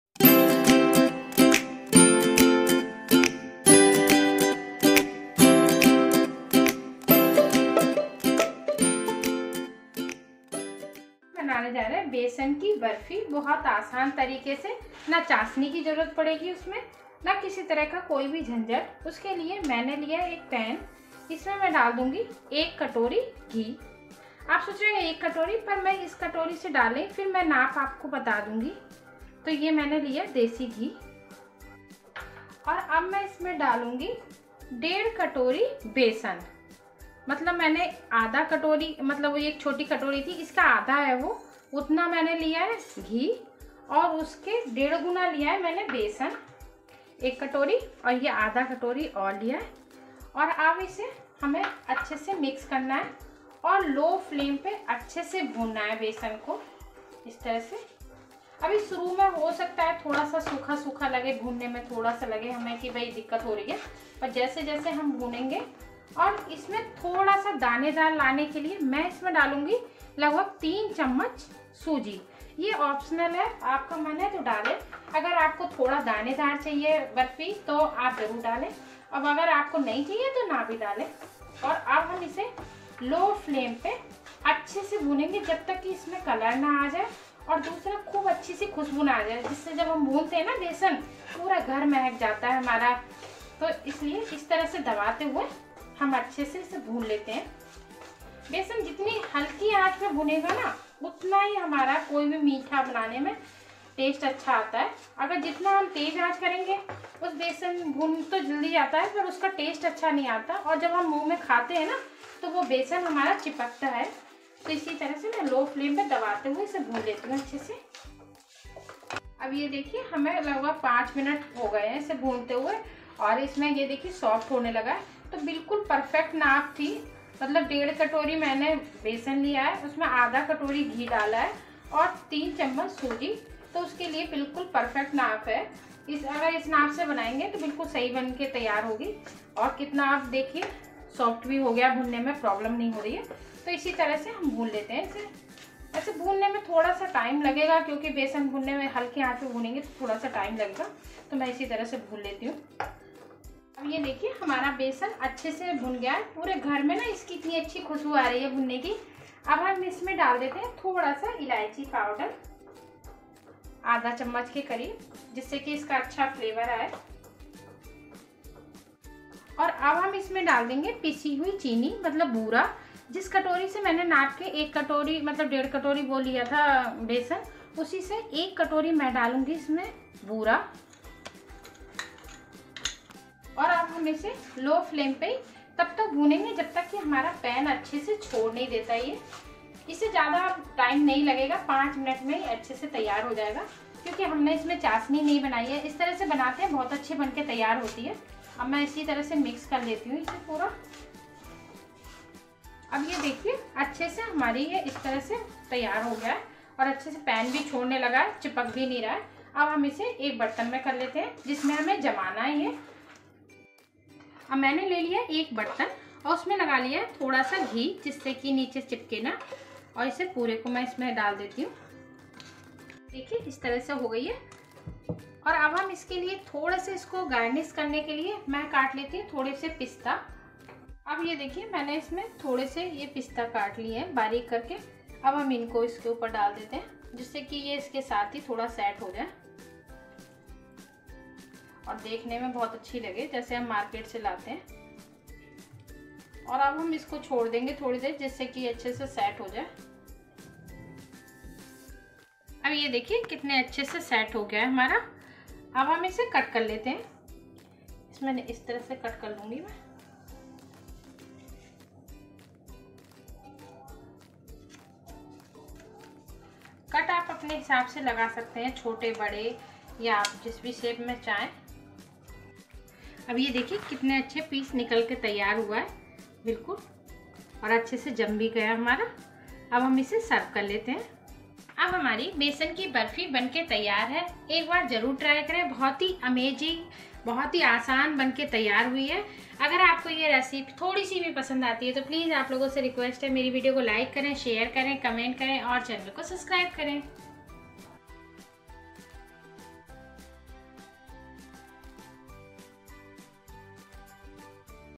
न मावा न चाशनी 10 Min में हलवाई जैसी दानेदर बेसन बर्फी | Besan Barfi Recipe
Ingredients:-

Besan :- 1 ½ Cup
Elaichi Powder :- ½ tspn
Sugar Powder :- ¾ Cup
Pista for Garnish